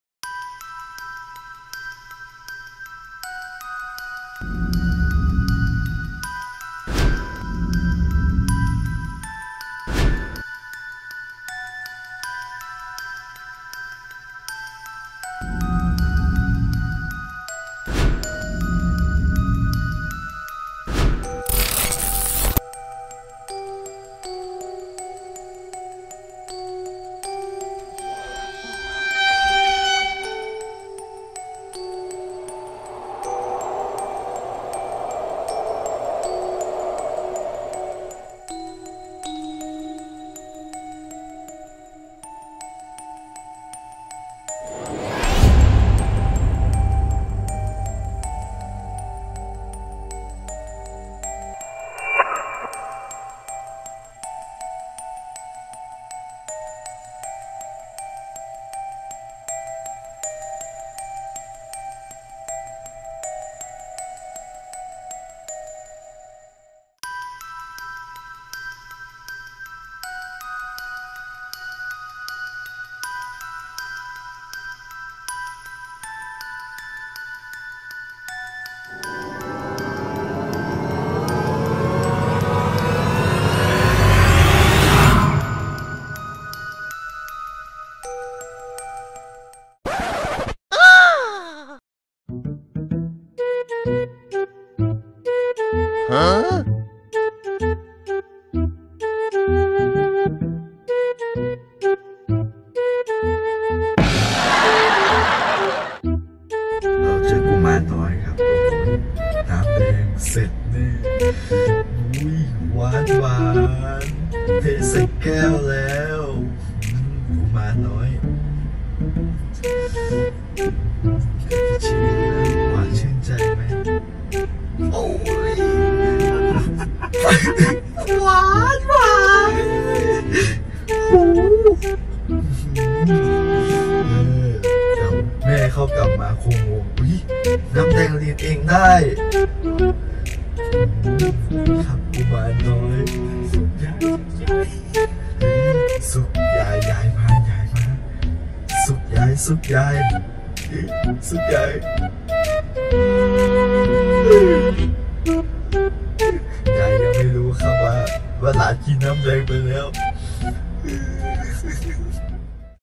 กลางวันก็ไม่เว้น โดนเล่นเข้าให้แล้ว
กลายเป็นคลิปที่กำลังได้รับความสนใจอยู่ในตอนนี้ หลังจากที่มีผู้ใช้ TikTok ชื่อว่า tk_t1_k1 ได้โพสต์คลิปวีดีโอของเด็กหญิงรายหนึ่ง พร้อมข้อความที่ว่า กุมารทองน้อย แอบกินน้ำแดง ซึ่งมีคนเข้ามาชมคลิปจำนวนมาก